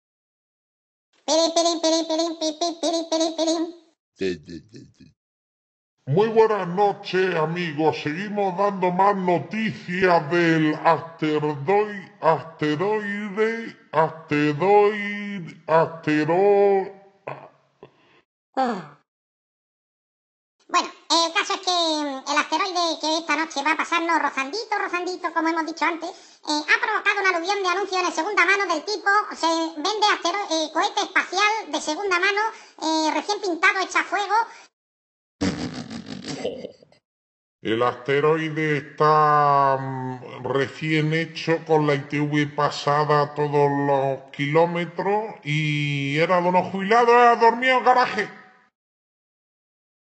Asteroide 2012 DA14 last news 2/3
Noticias de última hora sobre el asteroide que nos pasará muy cerca esta noche a las 21 y pico. Aumentan de forma masiva la compraventa de cohetes cosmosiderales de segunda mano